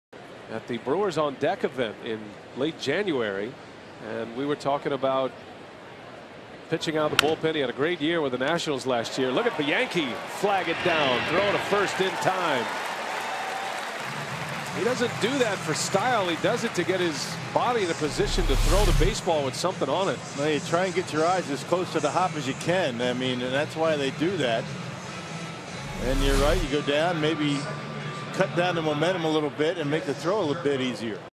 Bianchi takes a base hit away from Quentin
7/22/13: Jeff Bianchi makes a great diving stop on Carlos Quentin's grounder and fires across the diamond for the second out of the frame